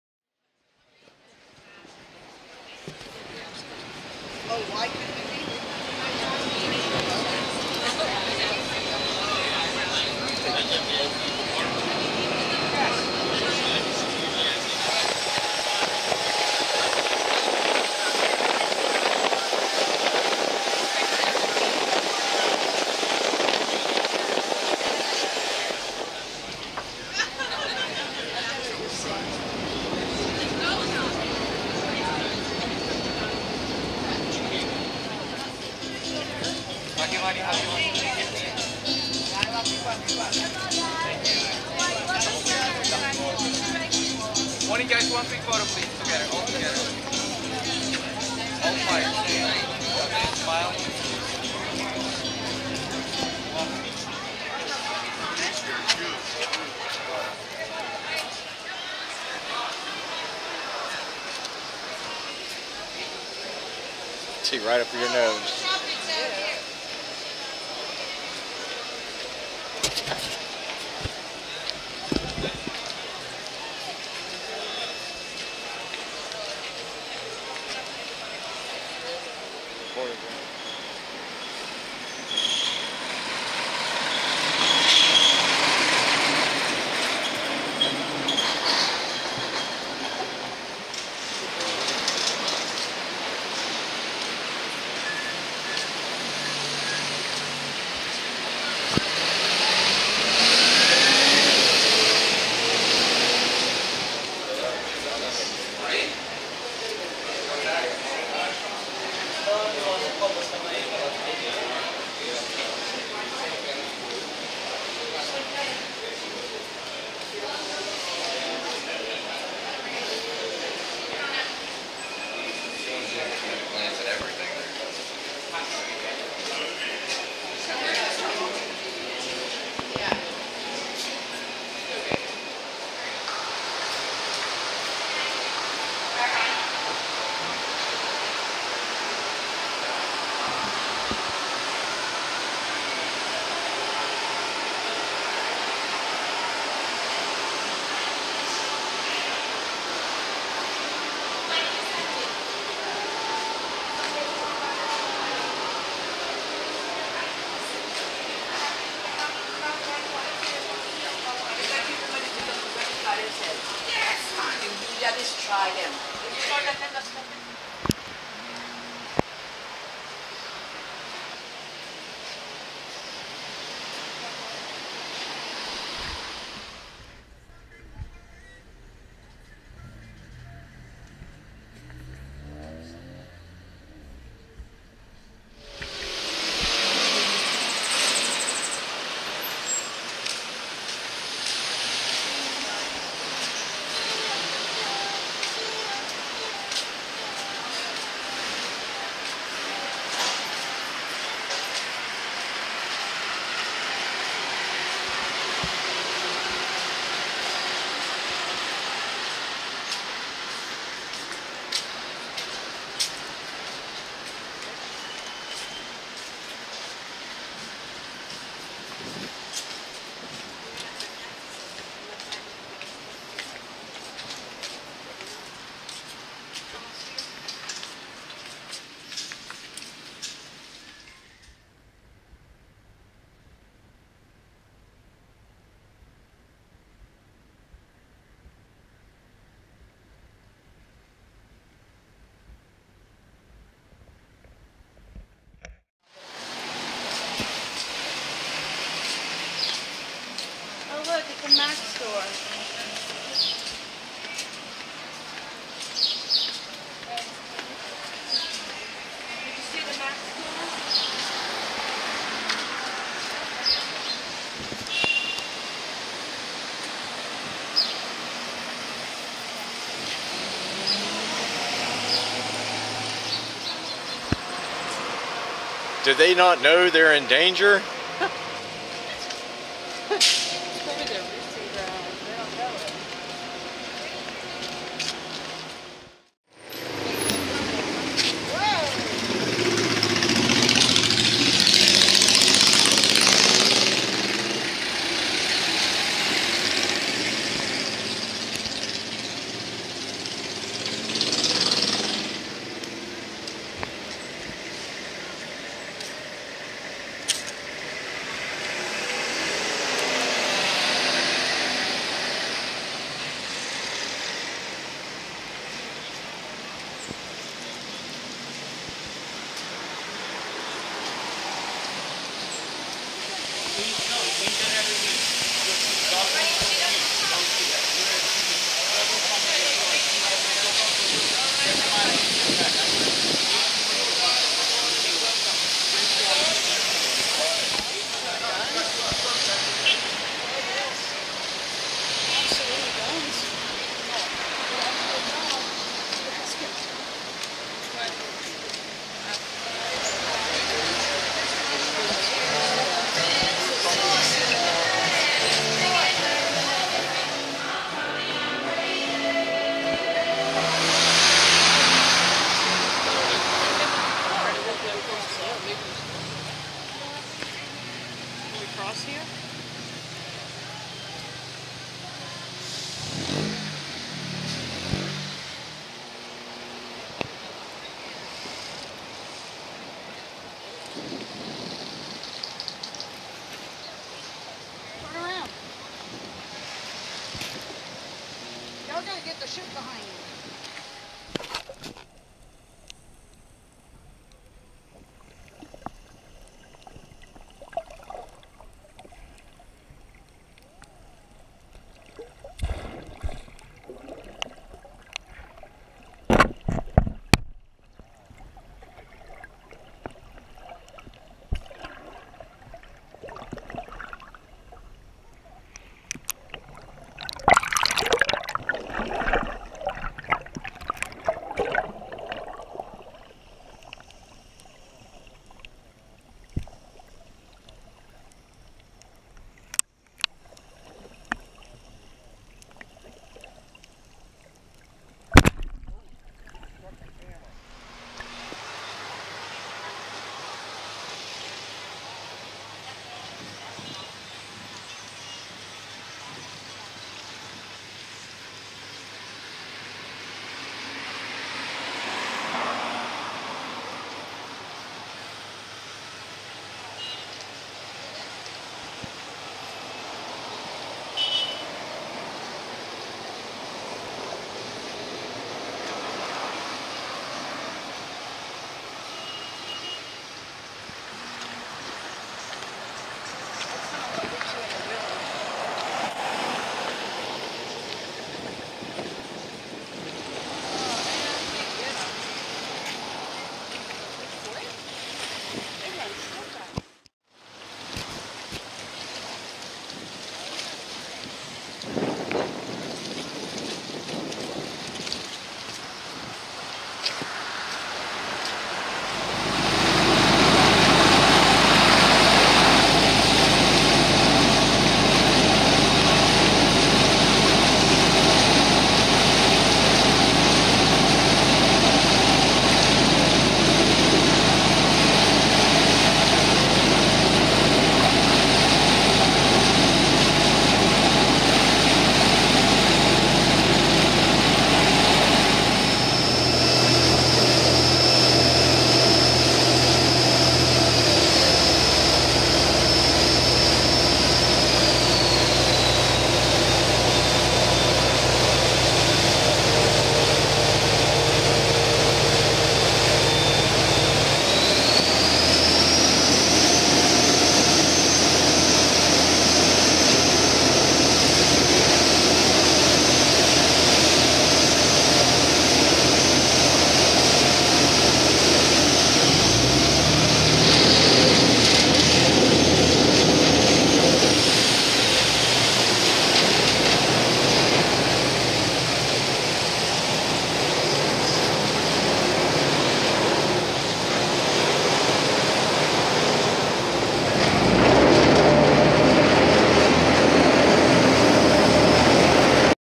Grand Cayman Shopping and sights.
video we made walking around Grand Cayman of the streets and shops that you will see there. We loved visiting there. While we were there we picked up some Big Black Dick (Caribbean Rum, Pervert) Did some shopping and had lunch at a small Mom and Pop resturant that overlooked the water. Great day.